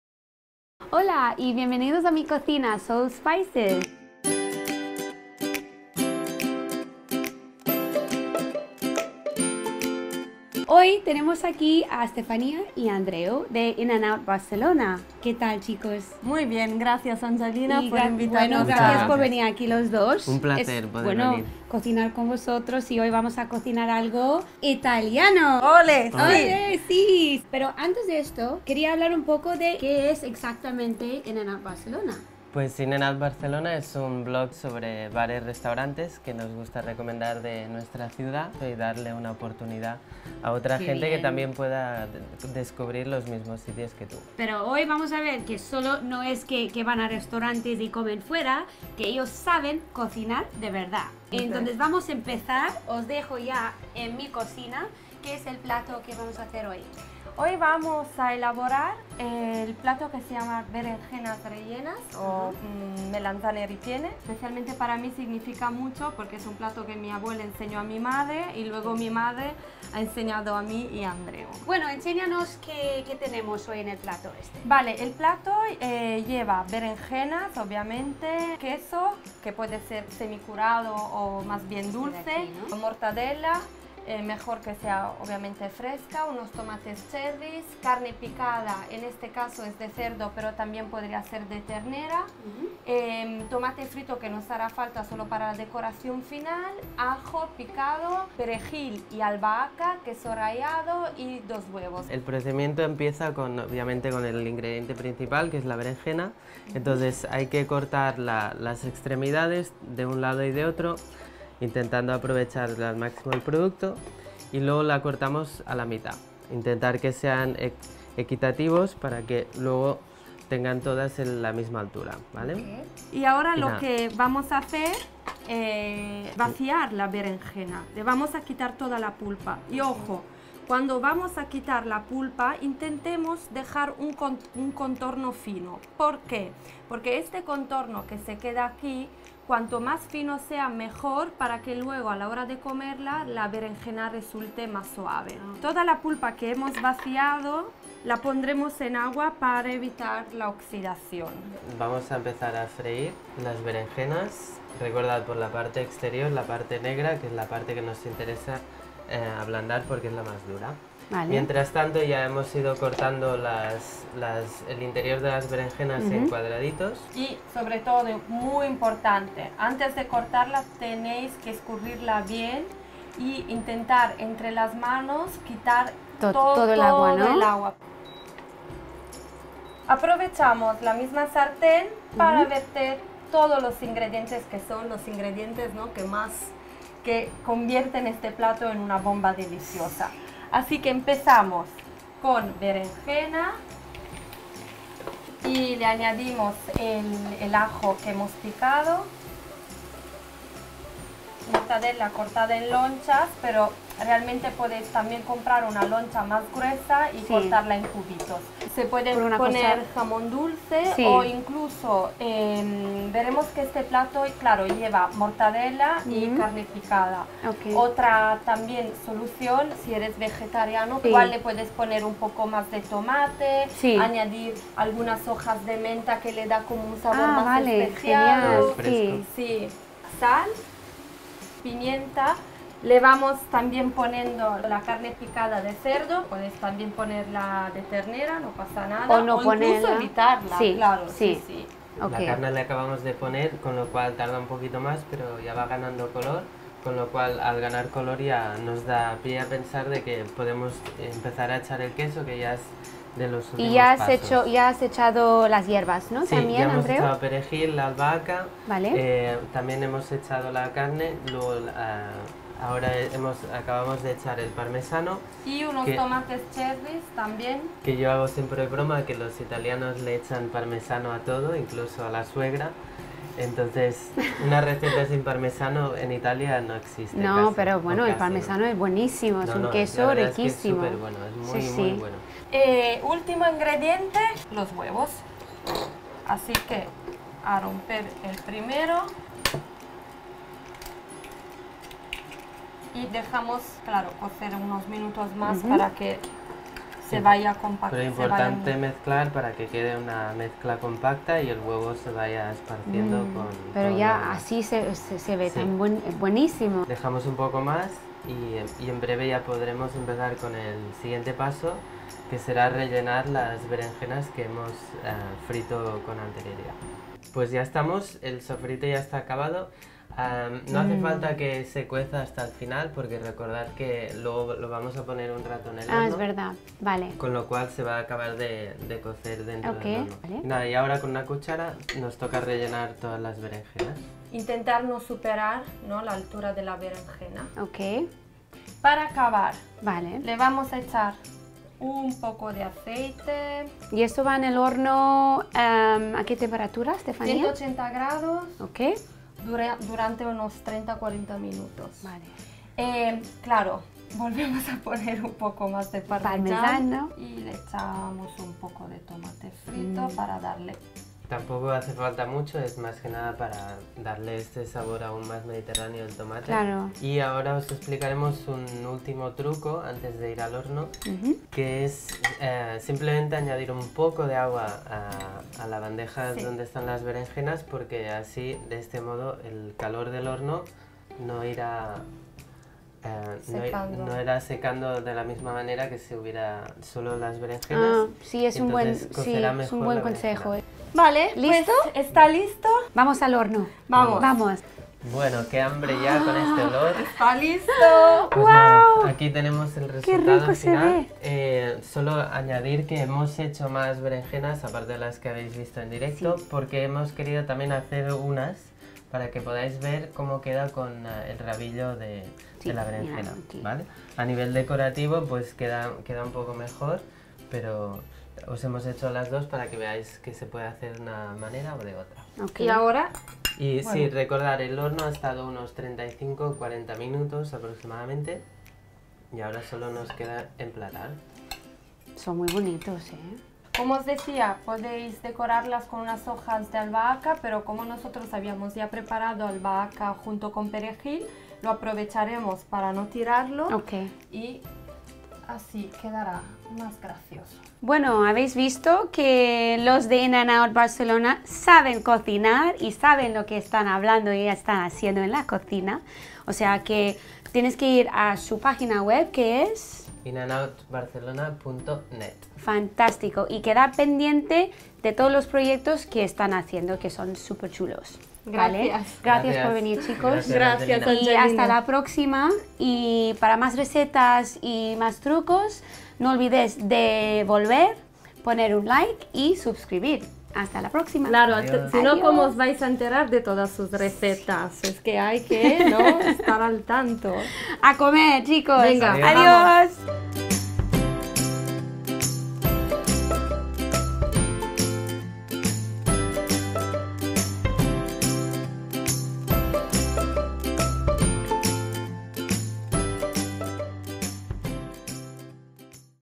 Receta de Berenjenas Rellenas In&Out Barcelona - Soul Spices - Anjalina Chugani
{English below} Hoy en Soul Spices Kitchen me visitan Stefania y Andreu de In&Out Barcelona, ¡una pareja 10!

Preparamos berenjenas rellenas, una receta 100% italiana que ha estado en el recetario de la familia de Stefania por generaciones. ¡Deliciosa! Y recordad, cocinar en compañía, ¡siempre es mejor!

Today in Soul Spices Kitchen we have a special visit from an amazing couple, Stefania and Andreu from In&Out Barcelona! They're showing us a 100% Italian recipe that's been in Stefania's Italian family for a while... A super fun day in the kitchen with these two proving that it's always better having company in your kitchen!